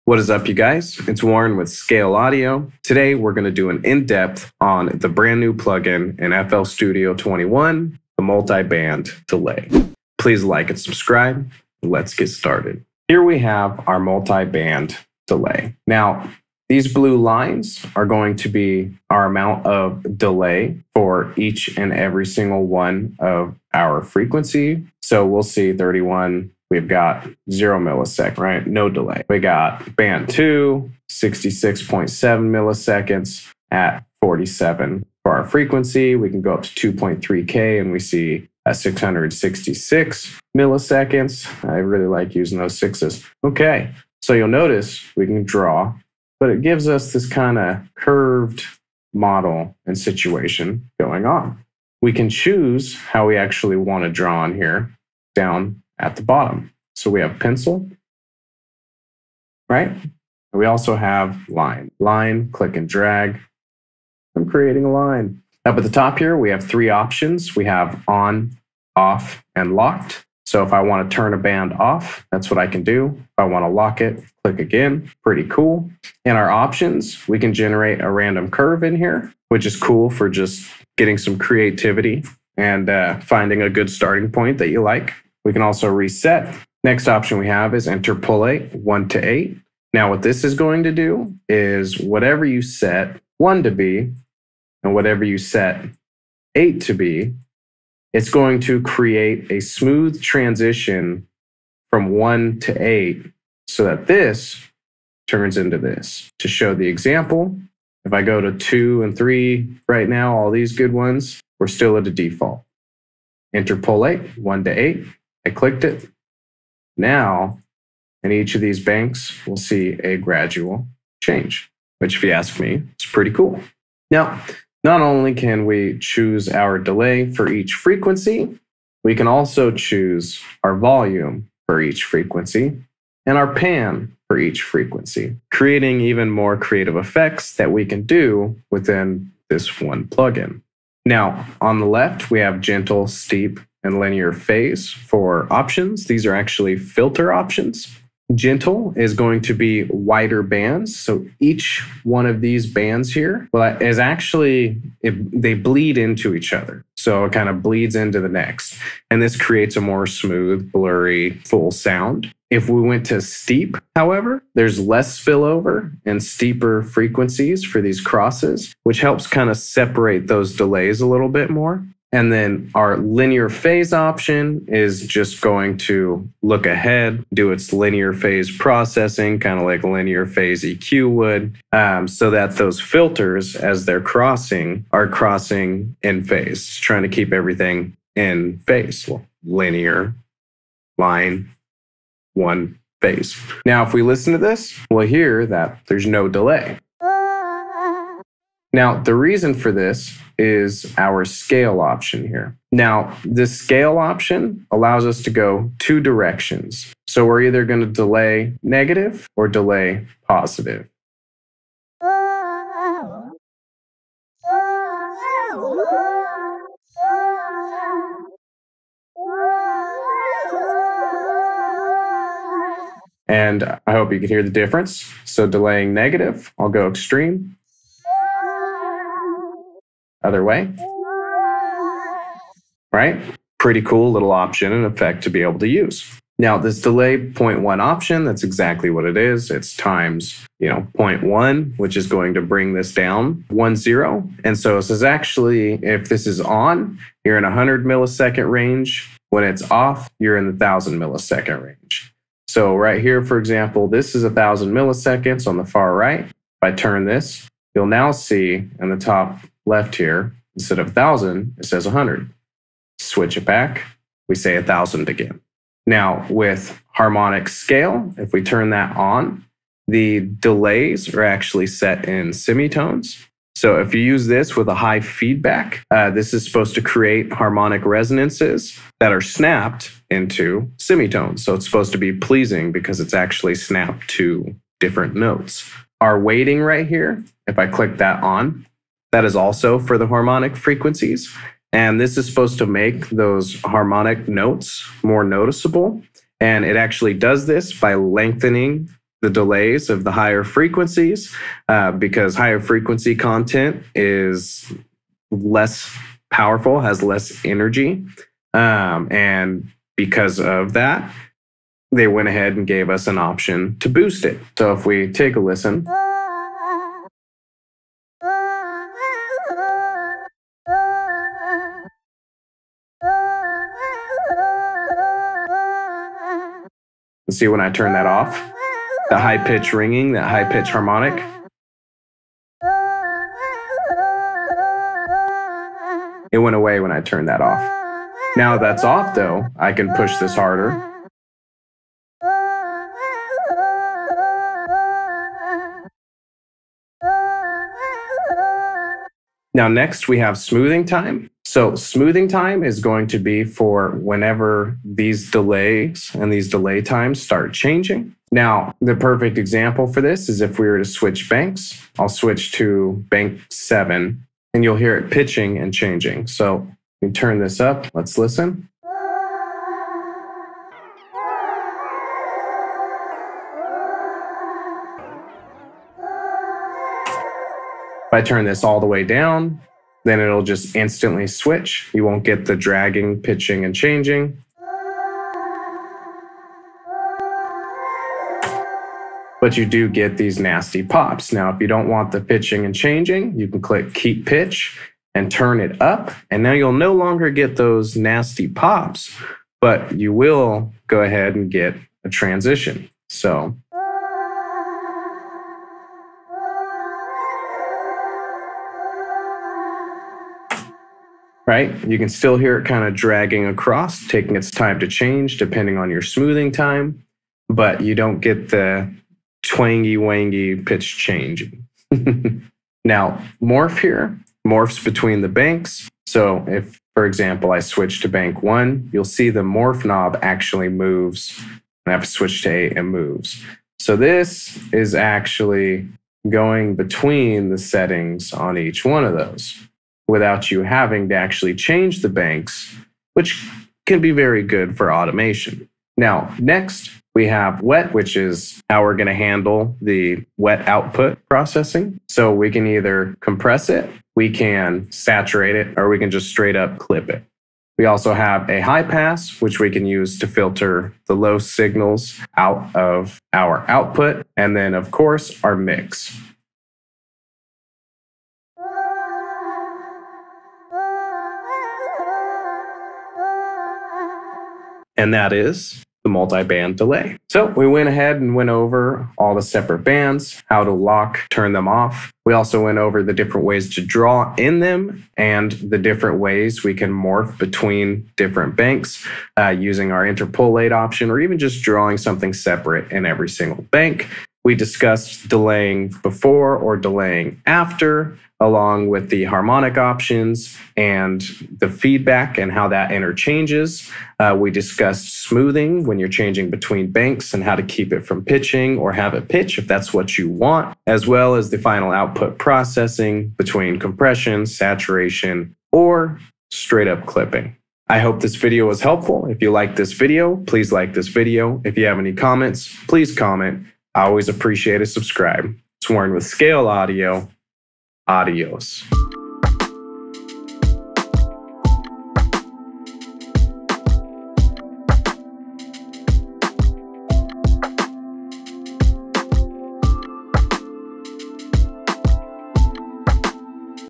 FL Studio 21 Multiband Delay (In Depth) | New Multiband Delay
The new Multiband Delay is here with the drop of the new FL Studio 21. FL Studio 21's Multiband Delay is a powerful tool that comes with FL Studio's Producer Edition. With different delay times available for multiple frequency bands, the Multiband Delay is a great addition to FL Studio's native plugins. With that said, let's take a deep dive into this "FL Studio 21 Multiband Delay" video.

👉Studio Monitors
👉Midi Controller
👉Interface (Mine is 1st Gen)
👉Dell G5 (Laptop)

⌚TimeStamps⌚
0:00 Intro
0:12 Multiband Delays
2:20 Filter Types
3:13 Scale and Feedback
5:49 Smoothing Time
7:02 Morphing
7:25 Wet HP and Mix
7:54 Summary/Outro